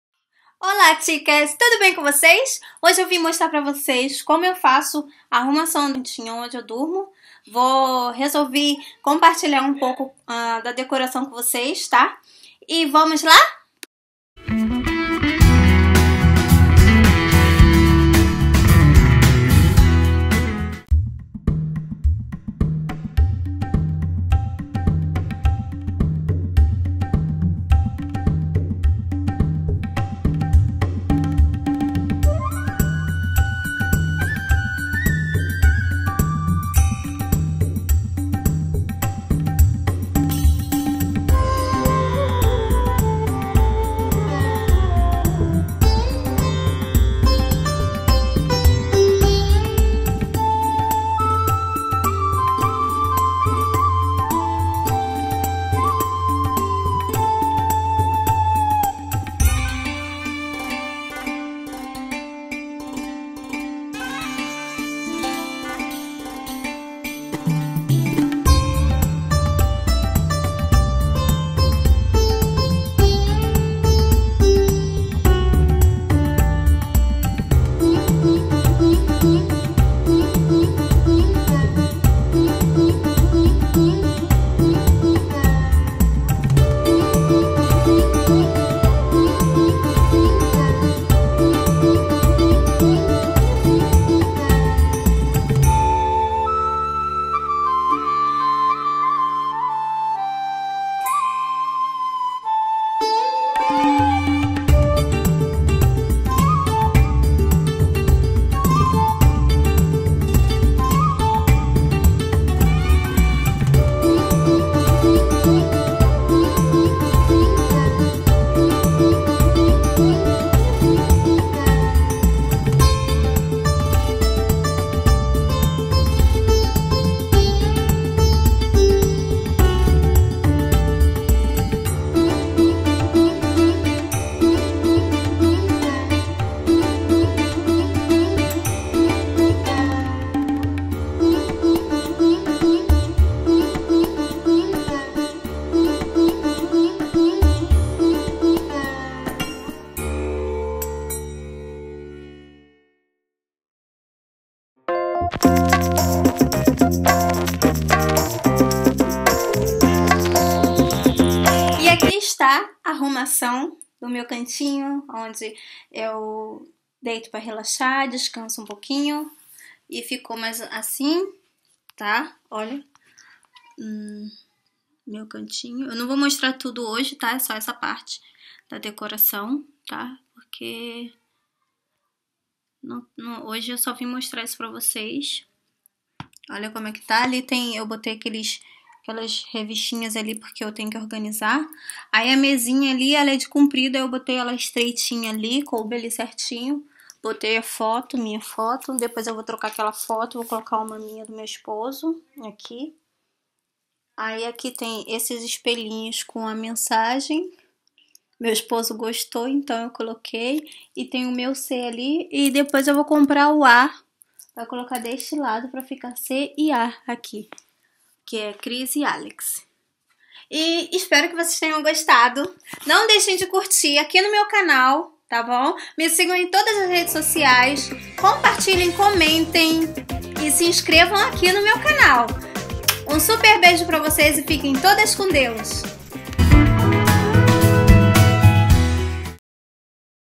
Re-Decorando um pedacinho do Meu Quarto (Re-Decorating my room)
Mostro um pouco de como eu faço nas minhas rotinas de decor na Minha casa. Blog Instagram Pinterest